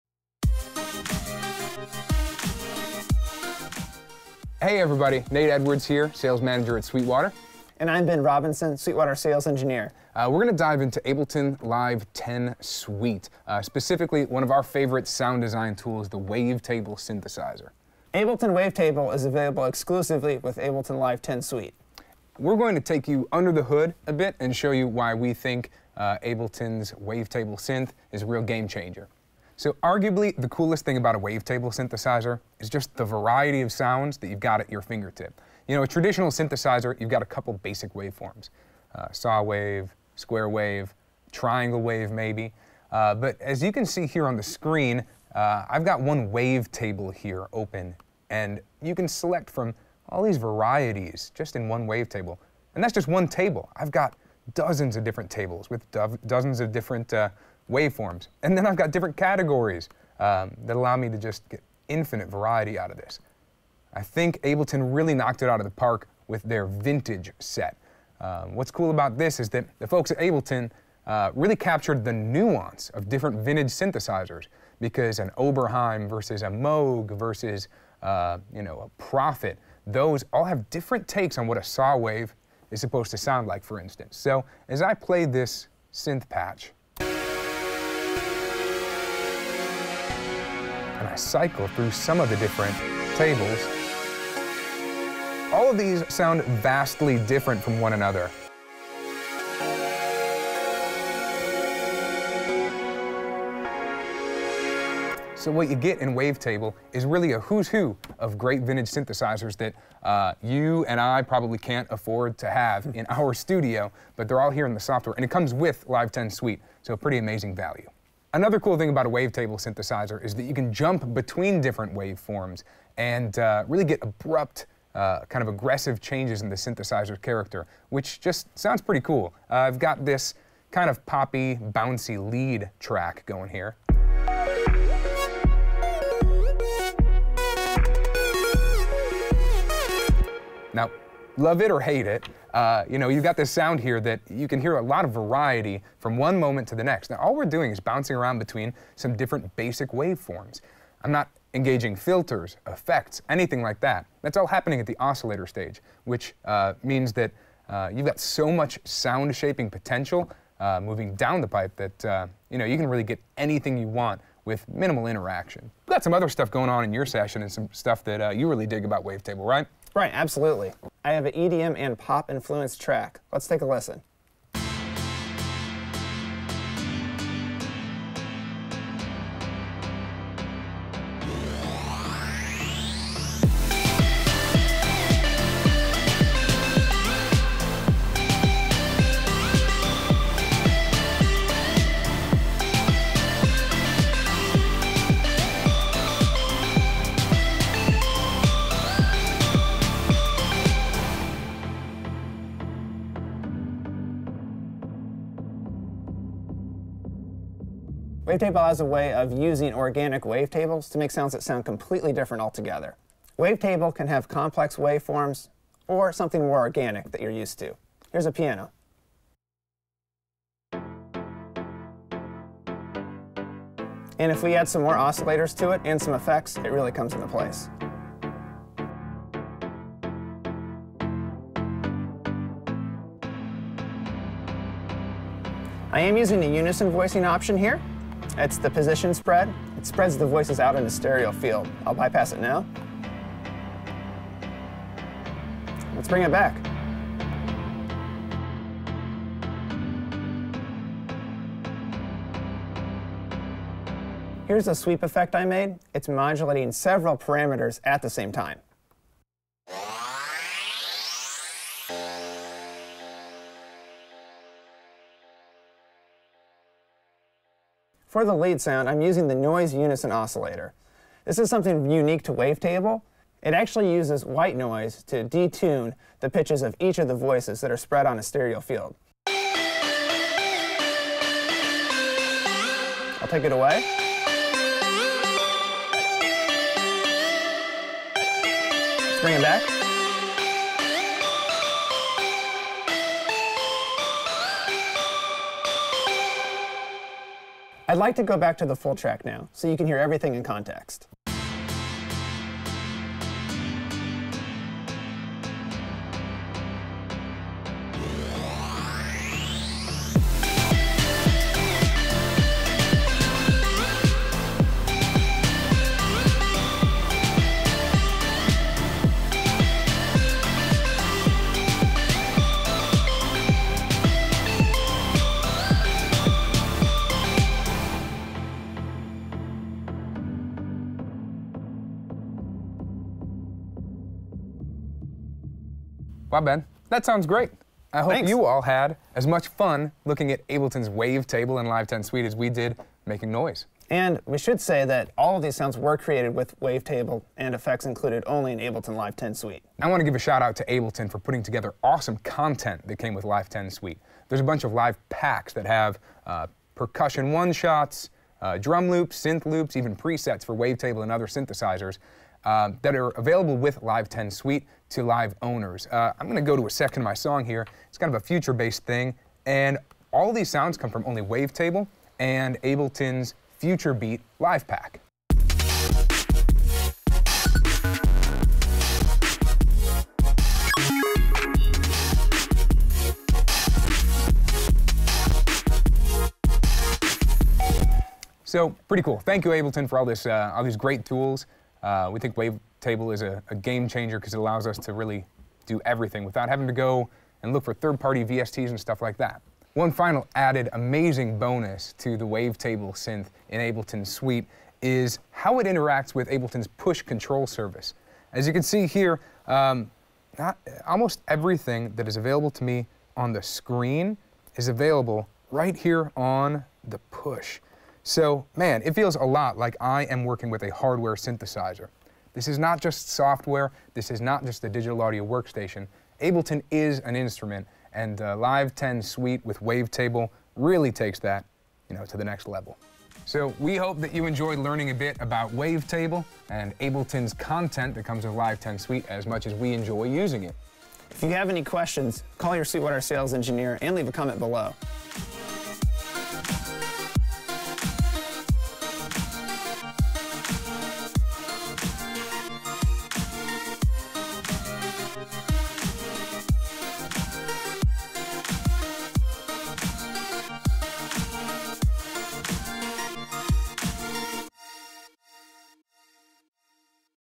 Diving into Ableton Live 10 and Wavetable
Designed completely in-house at Ableton, the Wavetable synthesizer features a wide range of waveforms, smooth analog-modeled filters, and comprehensive modulation options. Wavetable is capable of everything from familiar leads, pads, and bass sounds to etheric soundscapes.